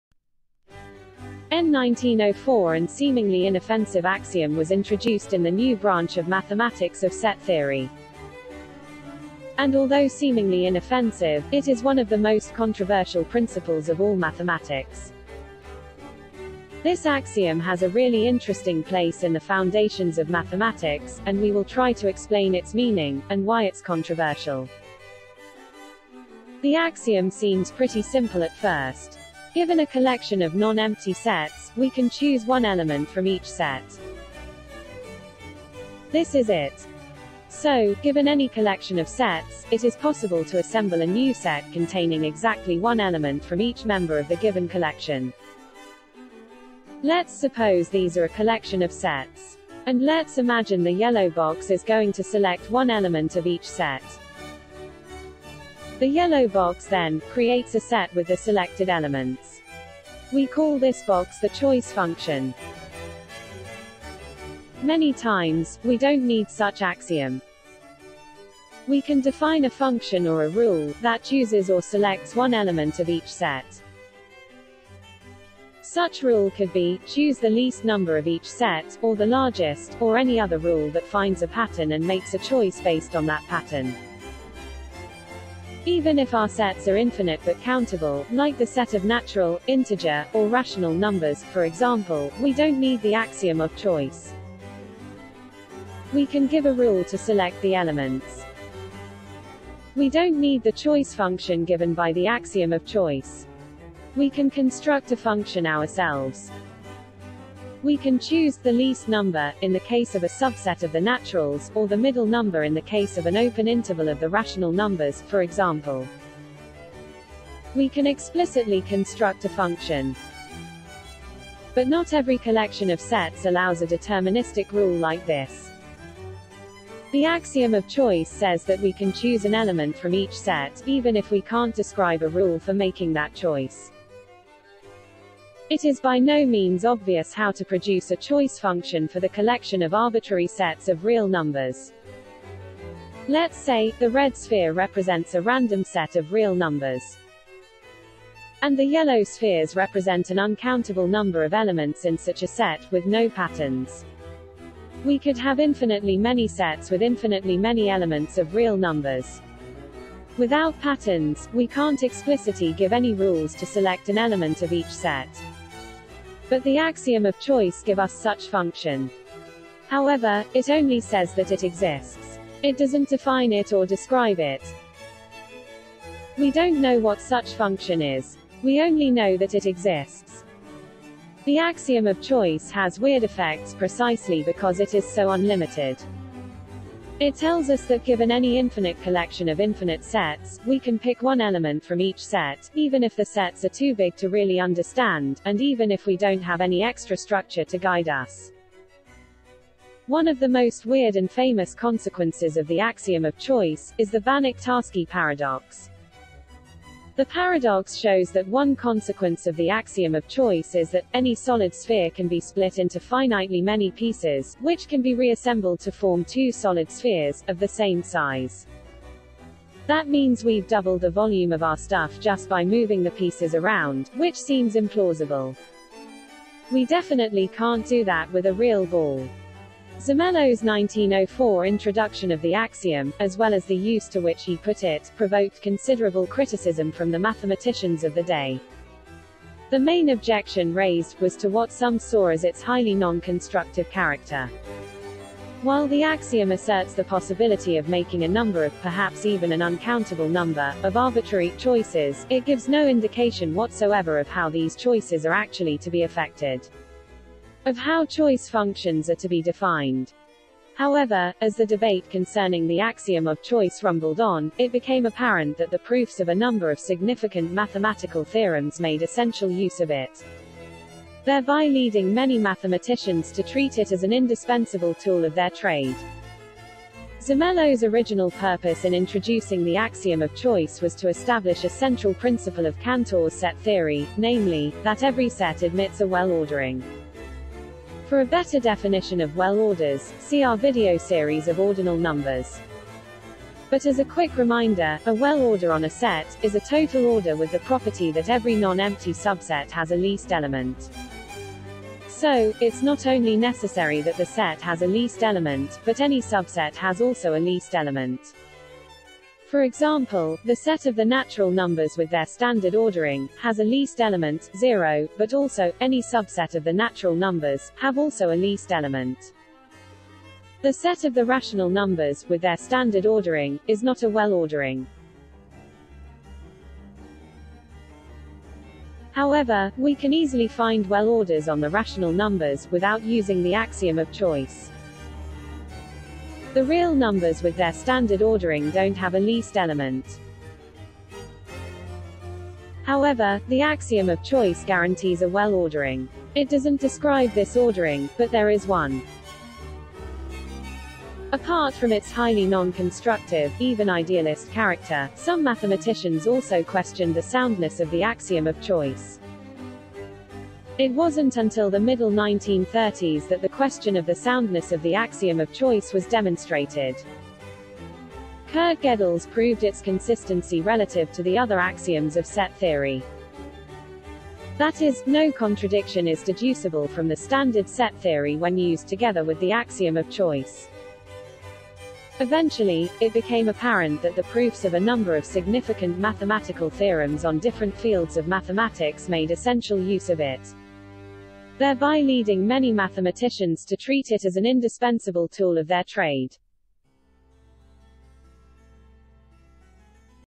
The Axiom of Choice.
In 1904 an seemingly inoffensive axiom was introduced in the new branch of mathematics of Set Theory. And although seemingly inoffensive, it is one of the most controversial principles of all mathematics.